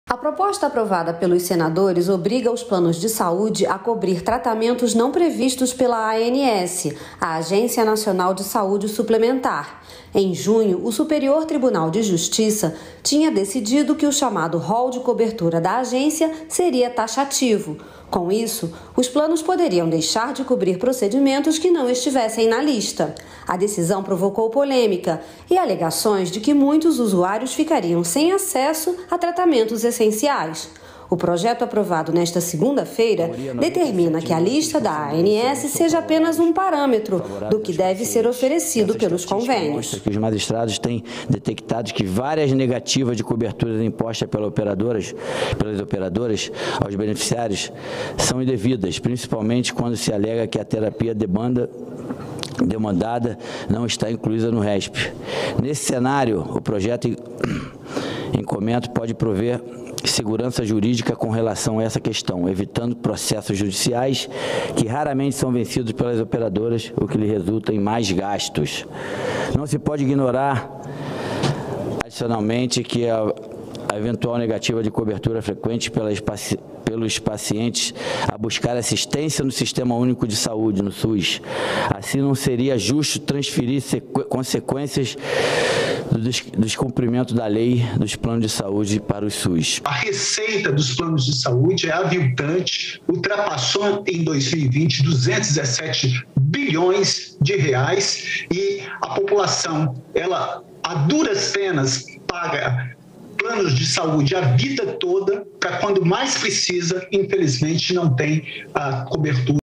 APROVADO PROJETO QUE OBRIGA PLANOS DE SAÚDE A COBRIREM TRATAMENTOS NÃO PREVISTOS NO ROL DA ANS
| BRASIL |